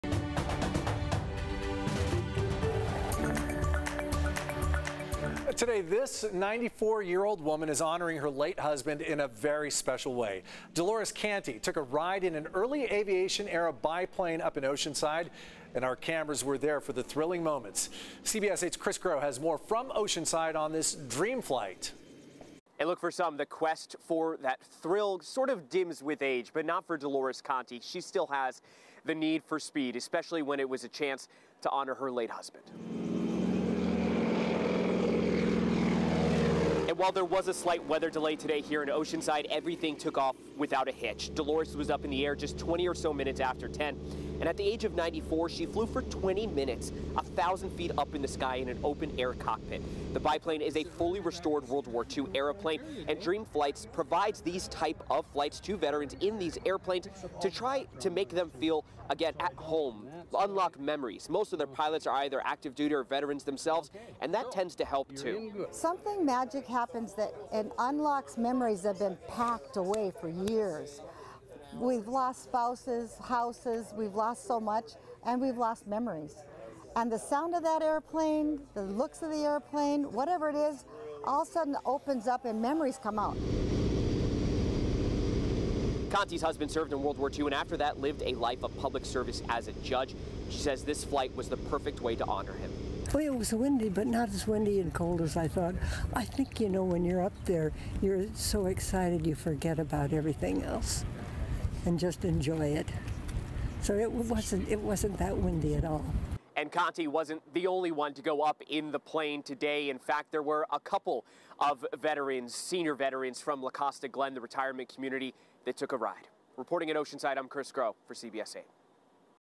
94-year-old takes flight to honor late husband
A 94-year-old woman honored her late husband on Thursday by taking off into the sky with a Dream Flight.

Dolores Canti took a ride in an early aviation-era biplane in Oceanside and CBS 8's cameras were there for the thrilling ride.

The dream flight was initially delayed due to weather. But eventually, Canti was able to liftoff without a hitch.